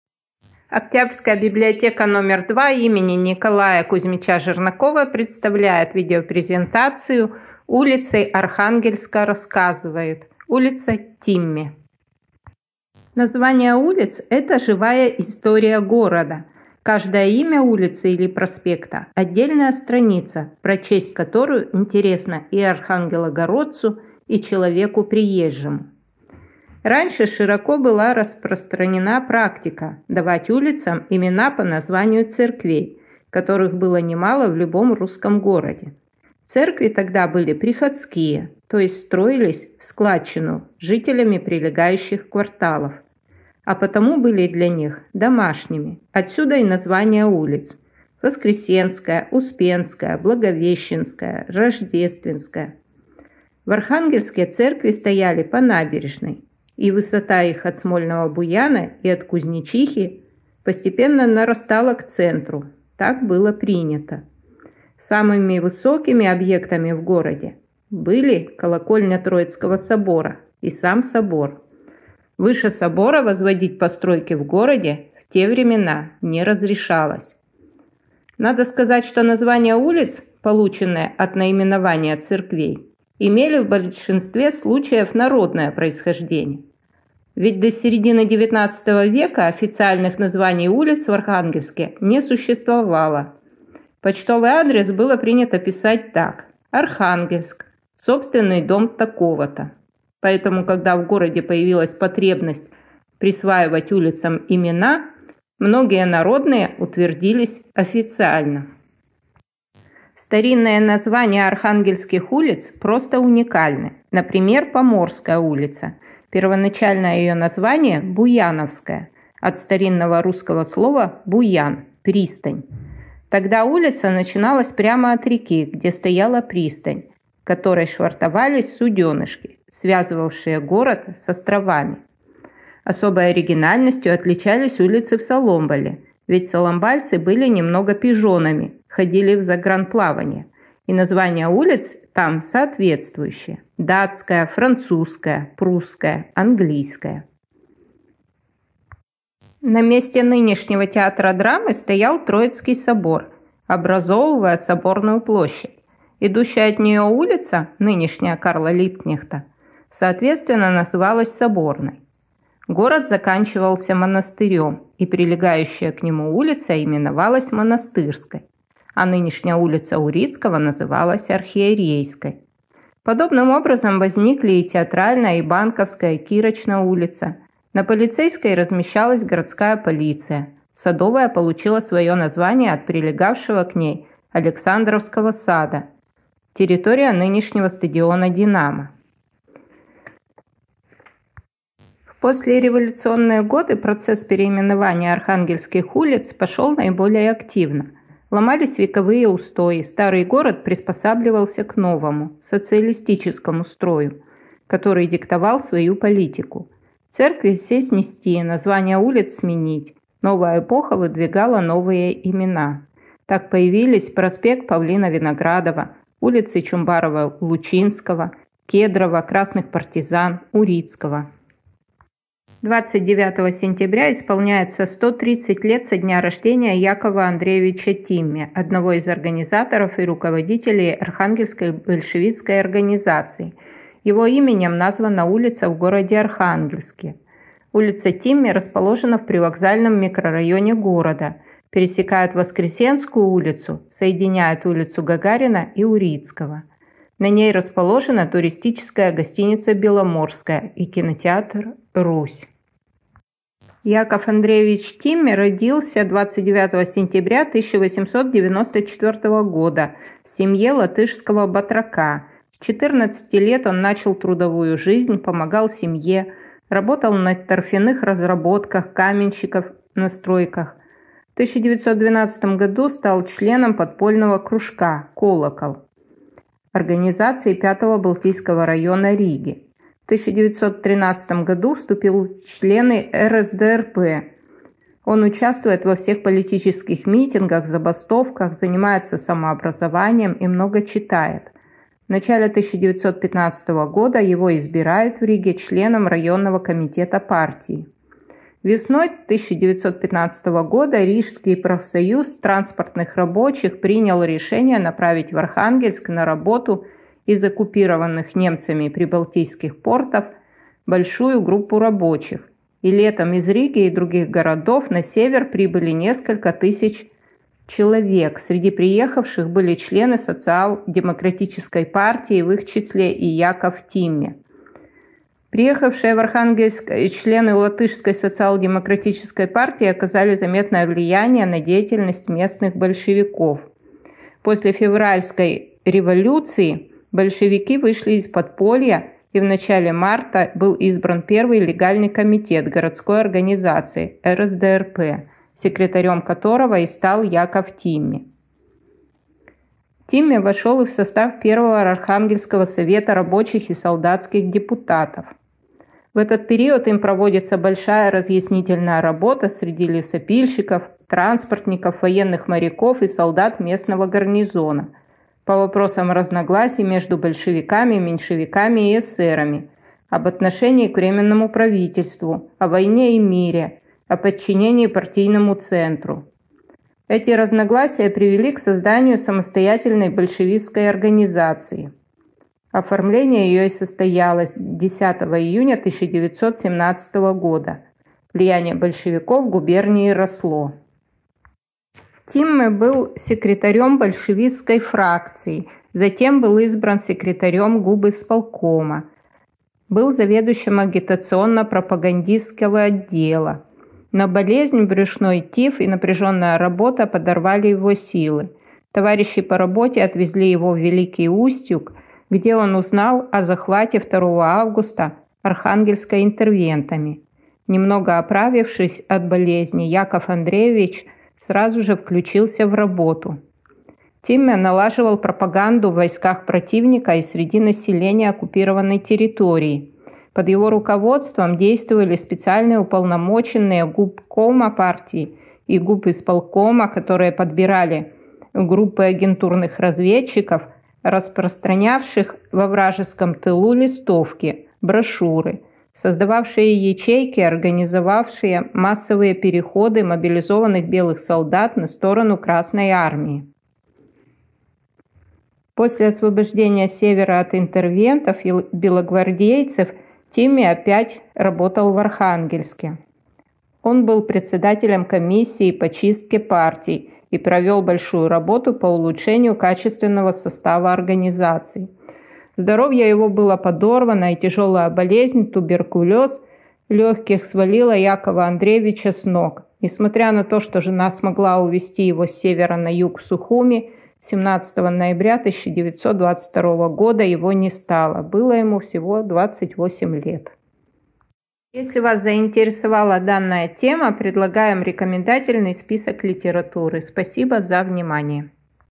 Улицы Архангельска рассказывают. Улица Тимме.
Сотрудники Октябрьской библиотеки № 2 им. Н. К. Жернакова рассказывают историю улицы Тимме в городе Архангельске, названную в честь Якова Андреевича Тимме, революционера и руководителя архангельской большевистской организации в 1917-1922 годах.